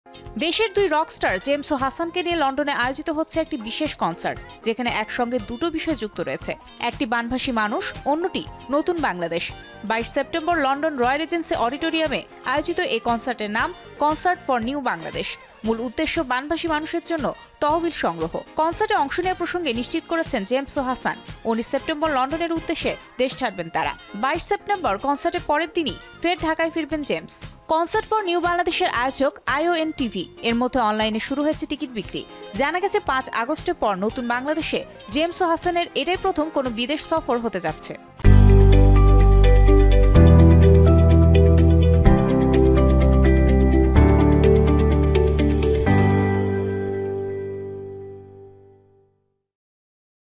লন্ডনে আর্ক-জেমসের ‘কনসার্ট ফর নিউ বাংলাদেশ’ | Concert for New Bangladesh | James | Ark | Jamuna TV
দেশের দুই রক স্টার জেমস ও হাসানকে নিয়ে লন্ডনে আয়োজিত হচ্ছে একটি বিশেষ কনসার্ট। যেখানে একসঙ্গে দুটো বিষয় যুক্ত রয়েছে। একটি বানভাসি মানুষ, অন্যটি নতুন বাংলাদেশ। ২২ সেপ্টেম্বর লন্ডন রয়েল রিজেন্সি অডিটোরিয়ামে আয়োজিত এই কনসার্টের নাম ‘কনসার্ট ফর নিউ বাংলাদেশ’!